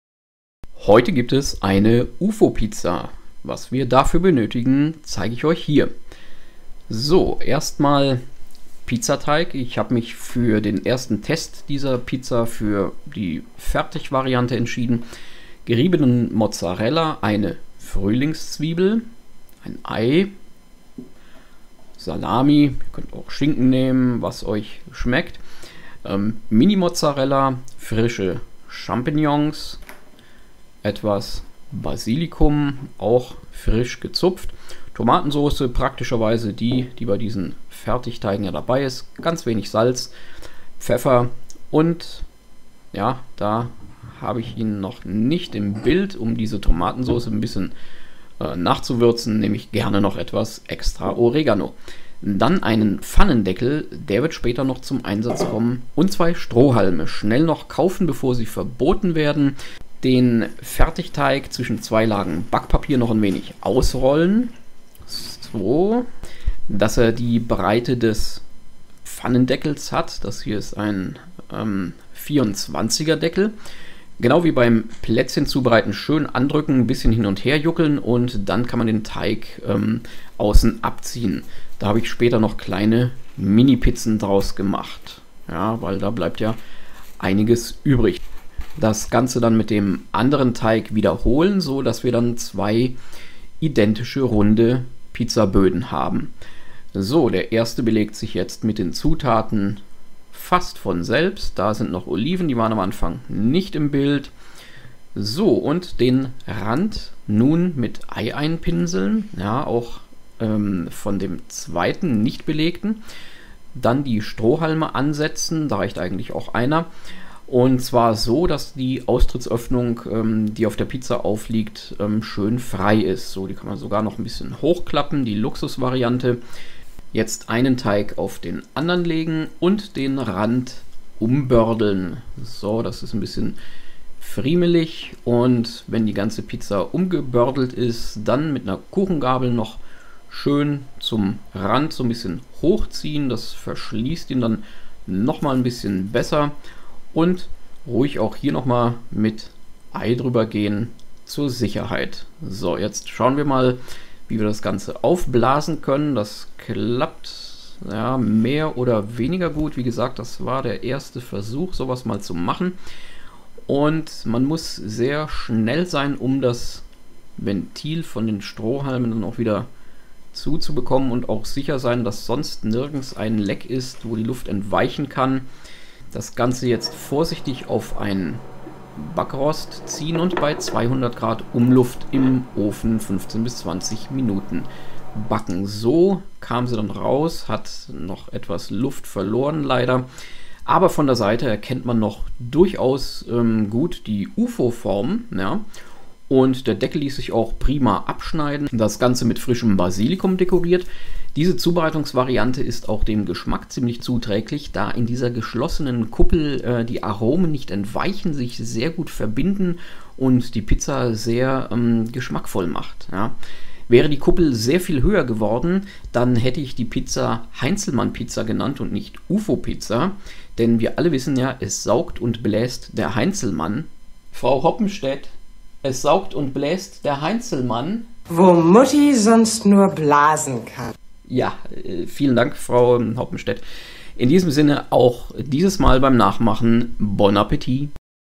Preppalots Rezepte: UFO-Pizza
Auf dem Videobild noch nicht zu erkennen - schaut einfach mal, wo oder wie sich das UFO in dieser Pizza wiederfindet. Ein originelles Rezept, an dem auch Kinder sicherlich viel Spaß haben, der mit selbstgemachtem Pizzateig bestimmt sogar noch größer wird - aber ich will nicht zuviel verraten... ;-)